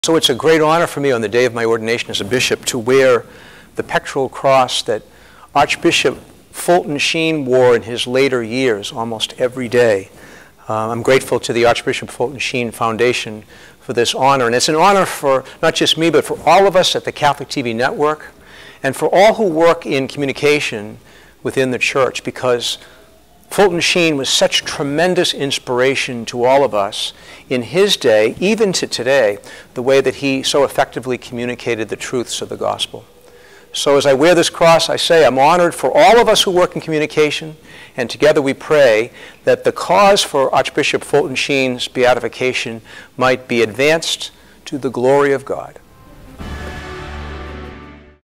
Archbishop Fulton Sheen's Cross | Bishop-elect Robert Reed
Bishop-elect Robert Reed shares about the great privilege of wearing Archbishop Fulton Sheen's Pectoral Cross on the occasion of his episcopal Ordination (August 24, 2016).